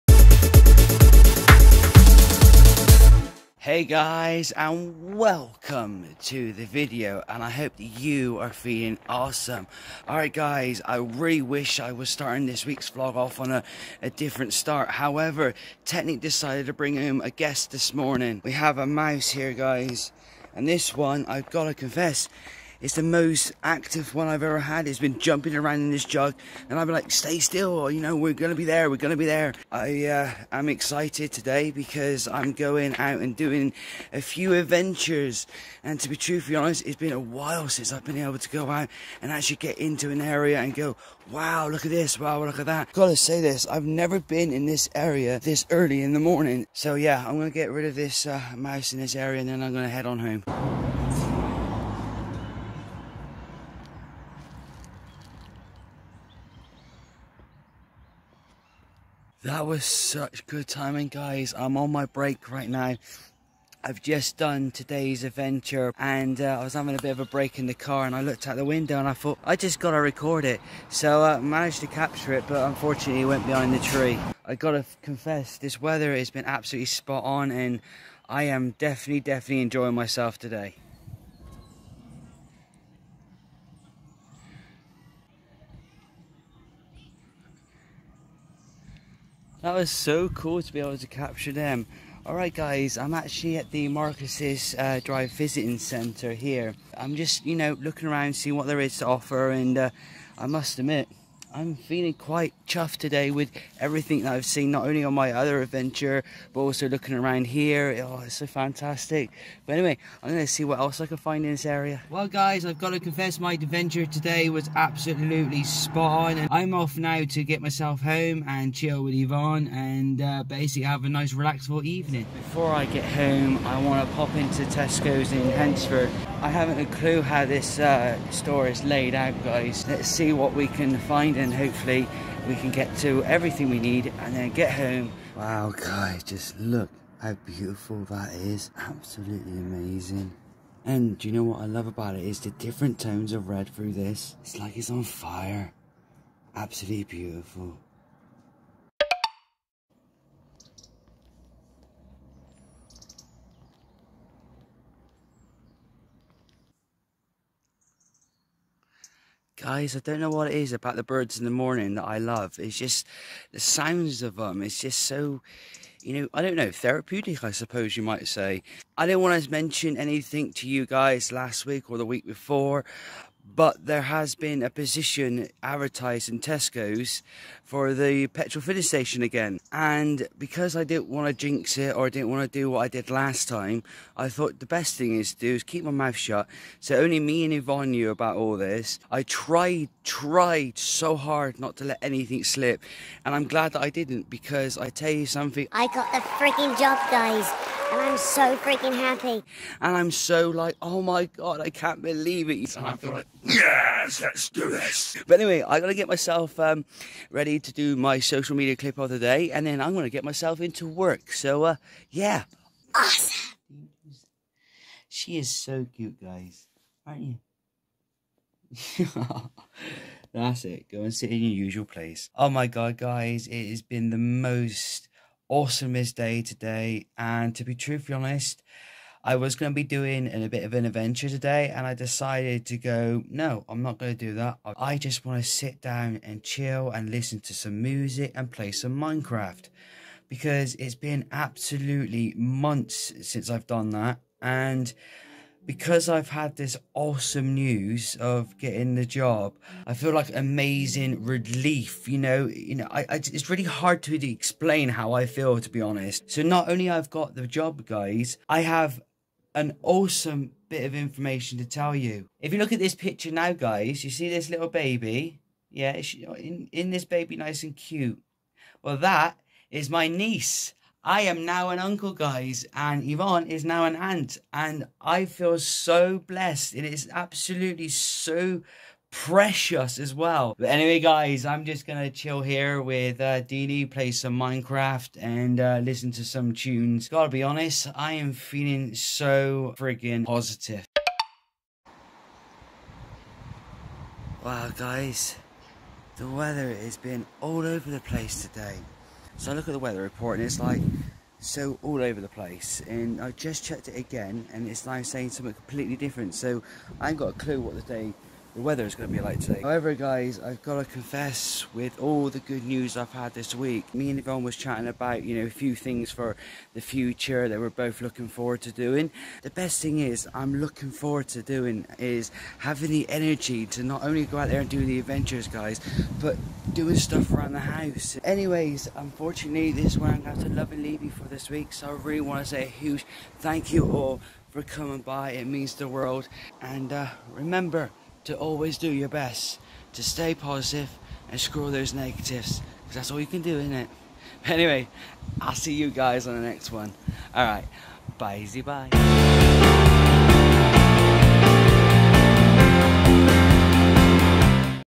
Jon's Weekly Vlog 50 It Finally Happened
On this weeks Vlog I was delighted to shared two bits of Fantastic News with you and I also shared some wildlife too.